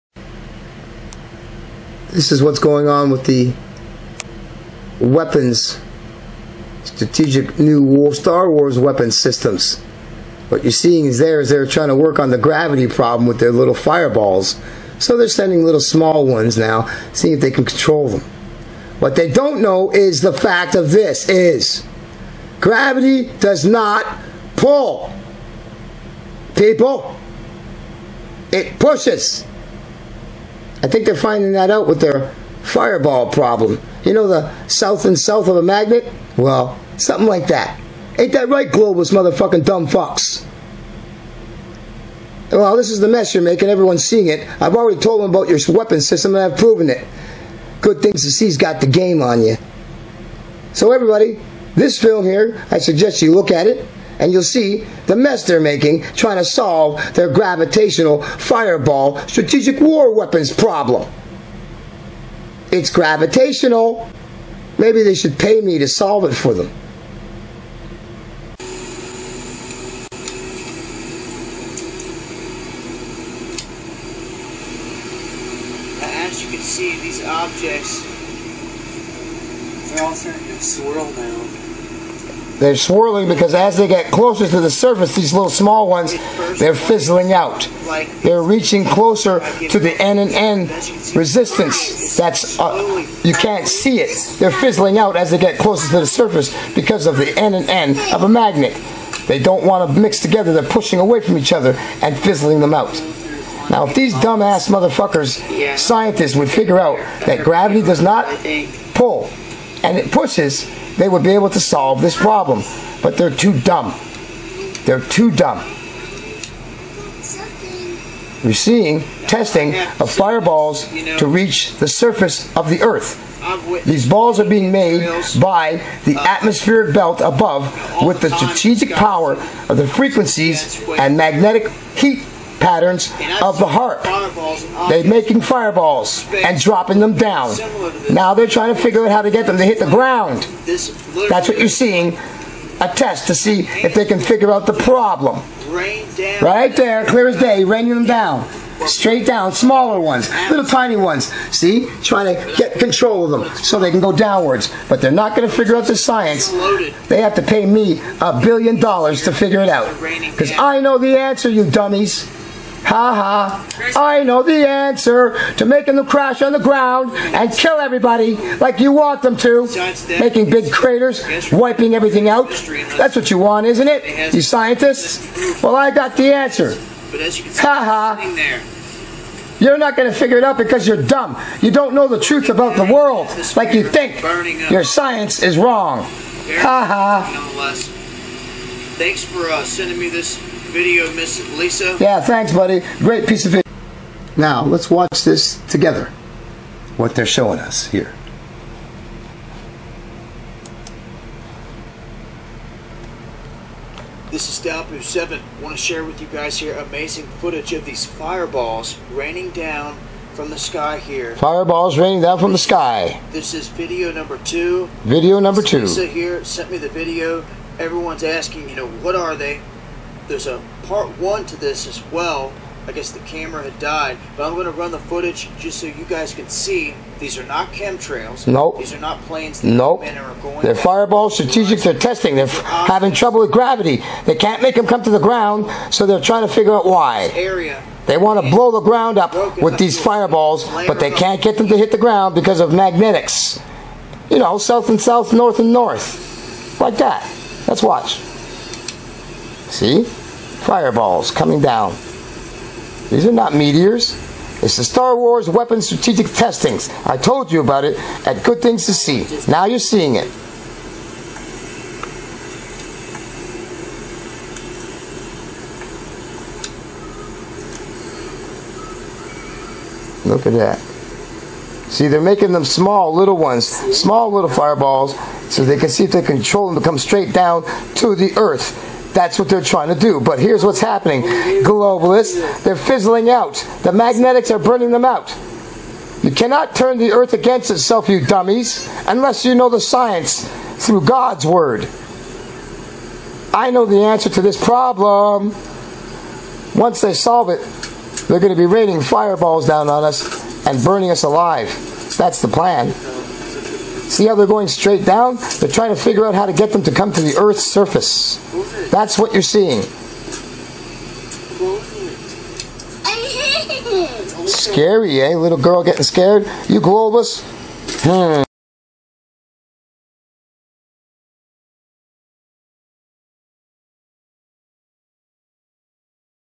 FIREBALL TESTING CONTINUES
GoodThings2c is aware of the Star wars weapons system  that is in testing phases at this time.
It is clear that we are knowing what real problem with their toy is that the fireballs will not hit
the ground as they want them to. They are Madmen Devils that control this machine.
THE HARRP  IN  ALASKA. We have the answer!!!